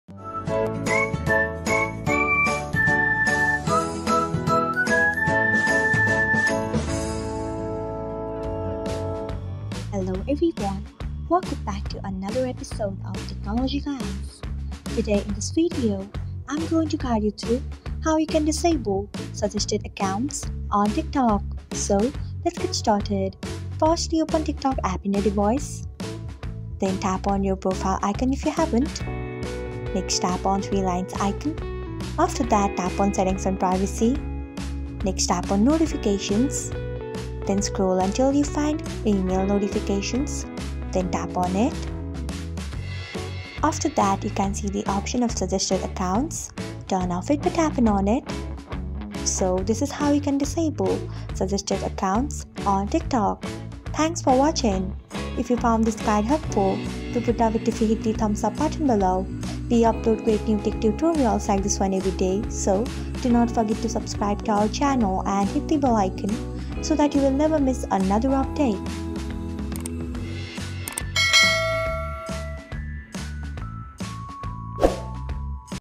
Disable Suggested Accounts On Tiktok. |Complete Step by Step Guide. |Technologyglance
Want to take control of your TikTok feed by disabling suggested accounts? In this tutorial, we’ll show you how to turn off suggested account notifications and recommendations, allowing you to enjoy a more personalized TikTok experience. By following our step-by-step guide, you can stop TikTok from recommending accounts to you, making your browsing experience cleaner and more focused. Watch now to learn how to disable suggested accounts on TikTok and tailor your social media experience!